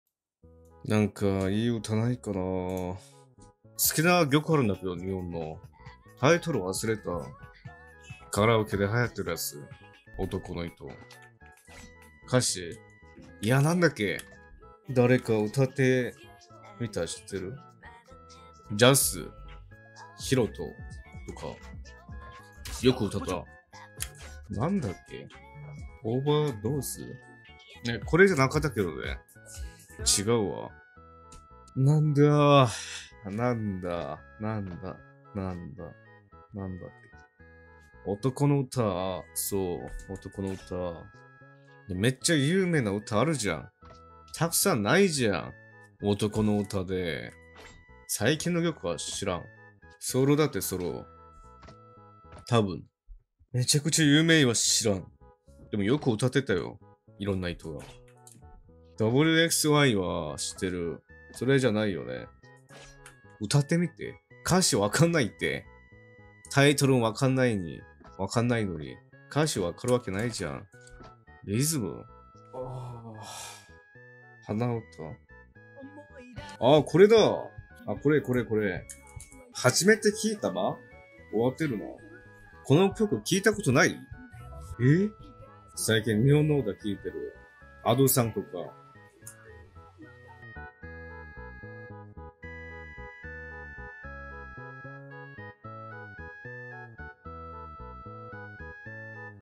【Mondo切り抜き】最近ハマっている日本のアーティストや曲について話すMondo【雑談/切り抜き】
音楽 : 魔王魂

チャンネルに関するご質問、ご意見はTwitterDMにて問い合わせお願いいたします。